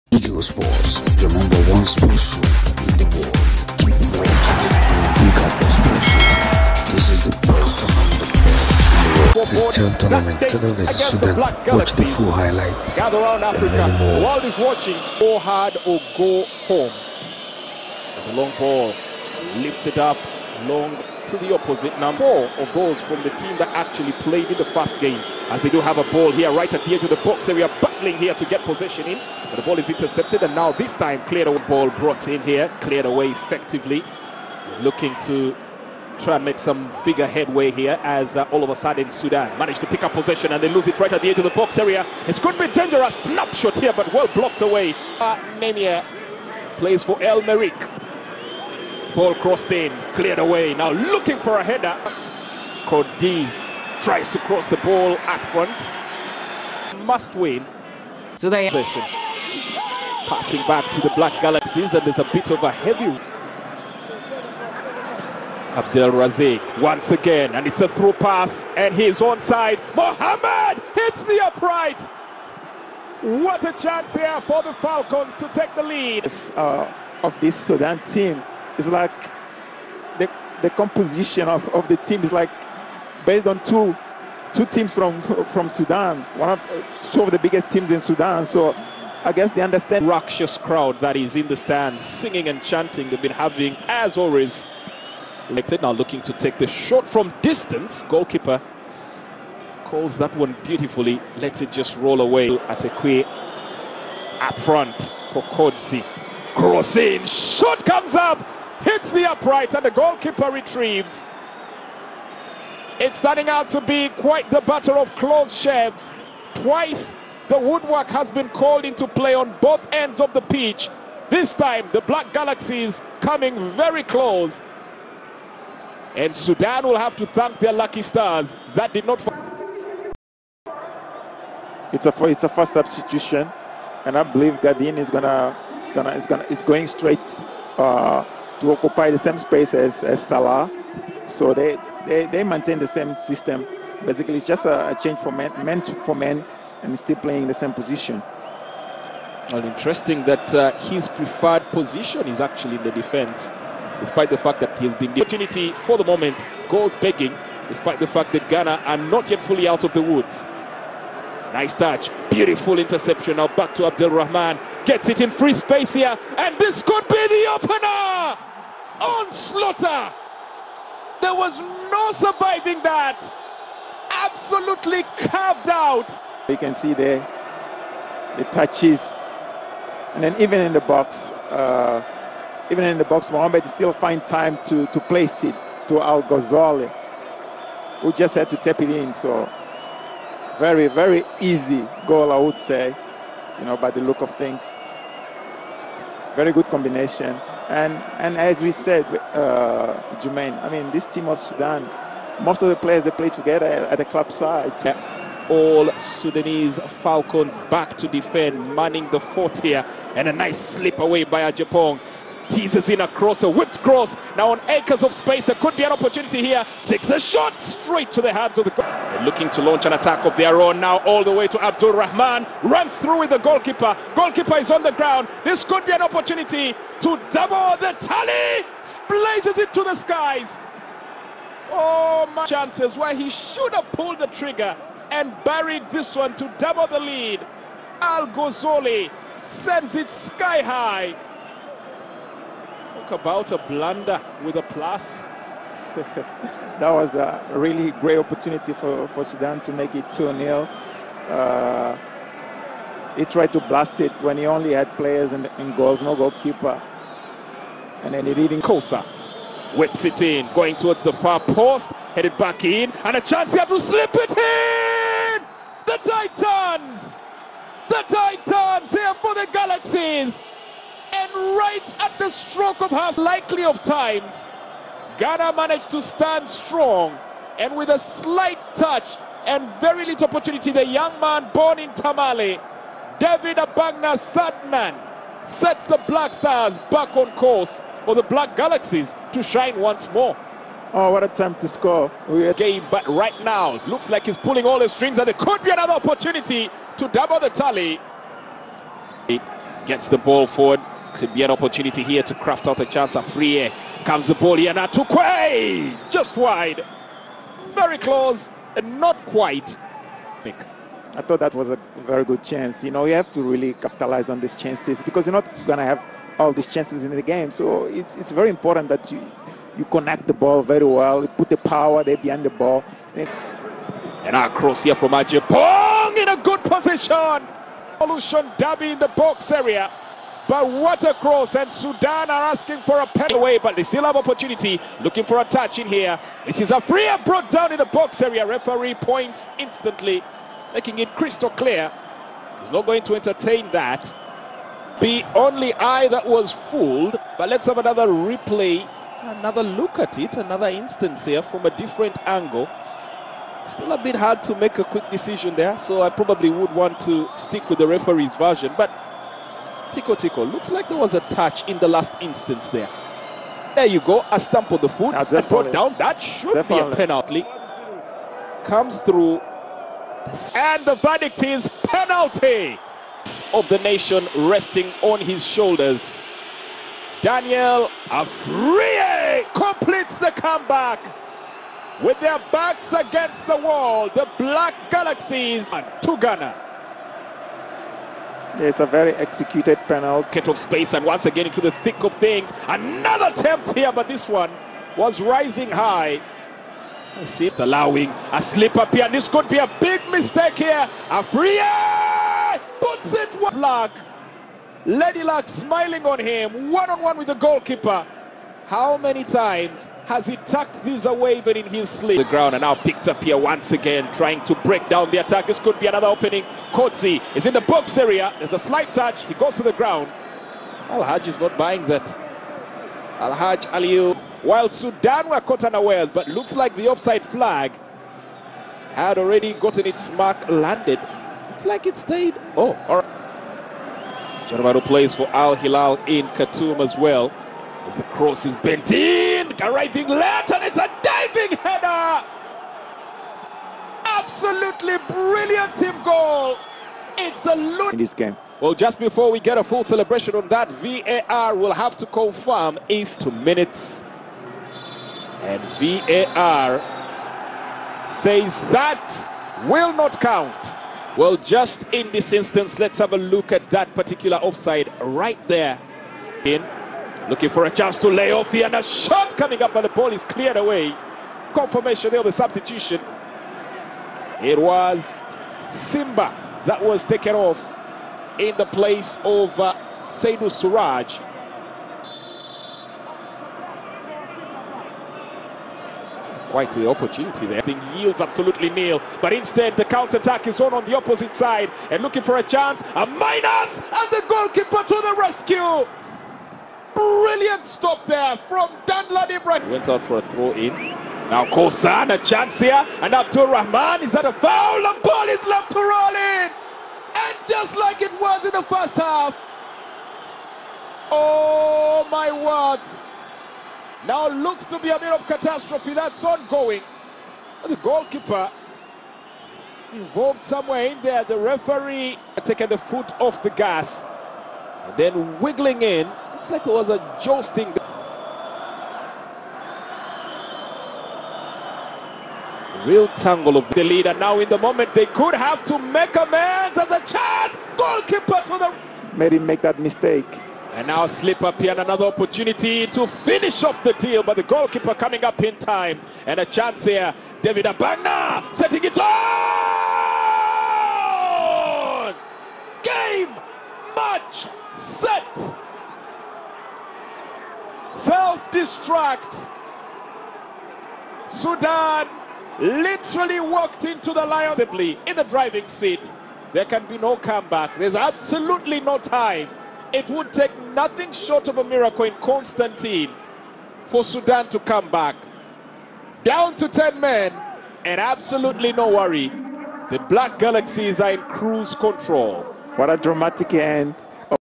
CHAN: Ghana 3 vs Sudan 1, Goals & Highlights
CHAN Tournament: Ghana 3 vs Sudan 1, Goals & Highlights, 2023, Constantine, Algeria